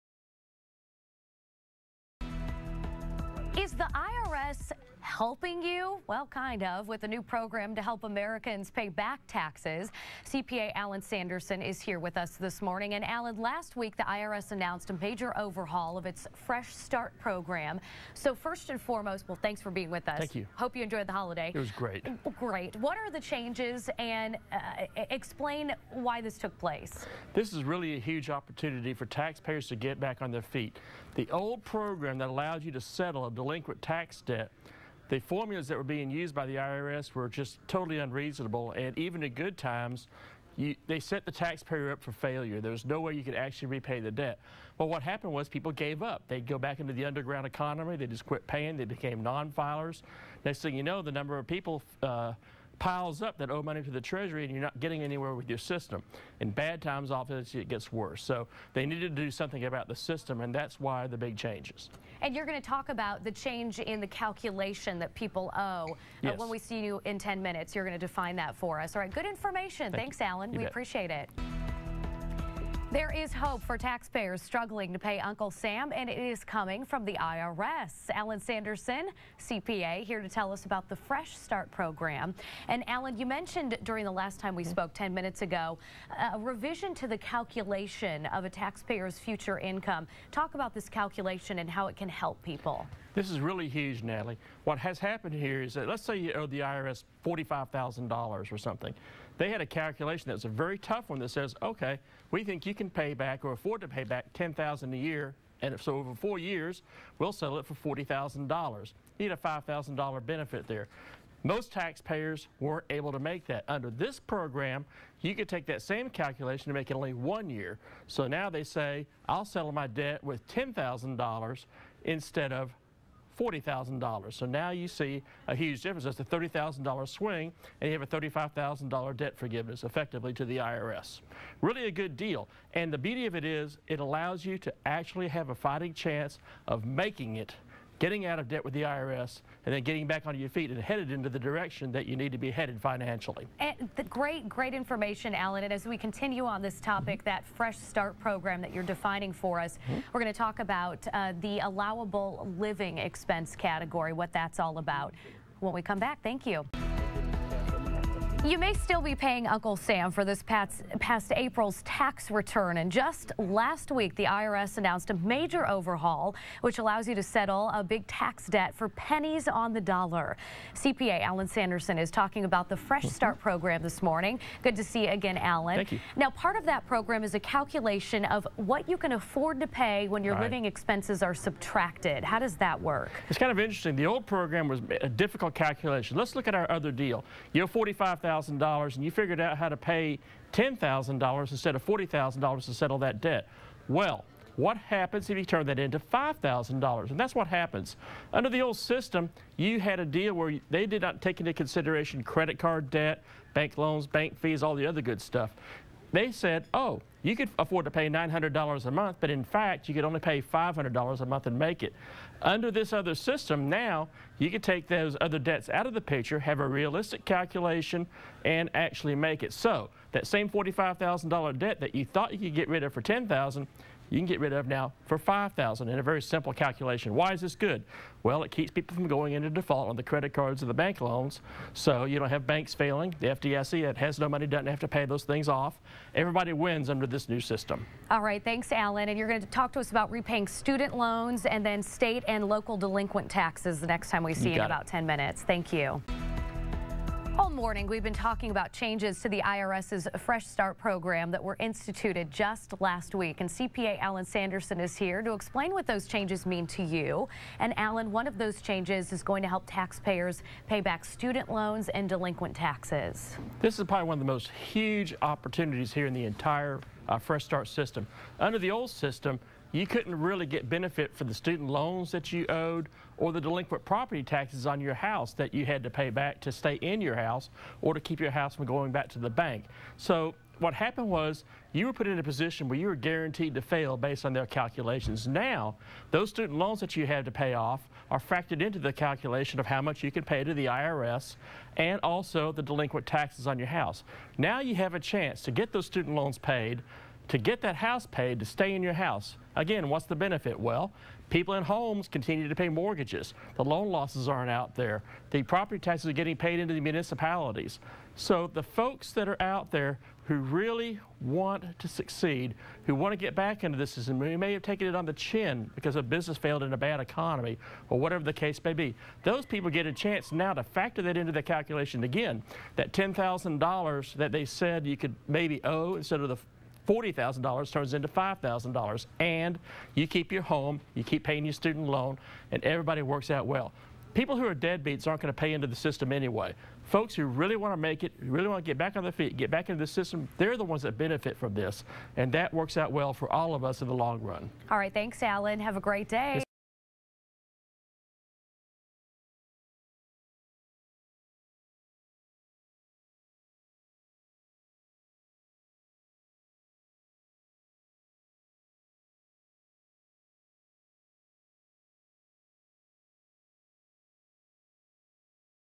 Alan Sandersen - IRS Help With Back Taxes
Alan Sandersen of Sandersen Knox LLP, Certified Public Accountants,  shares with KRIV Fox News anchor, Natalie Bomke about ways to negotiate with the IRS in regards to lowering your tax burden.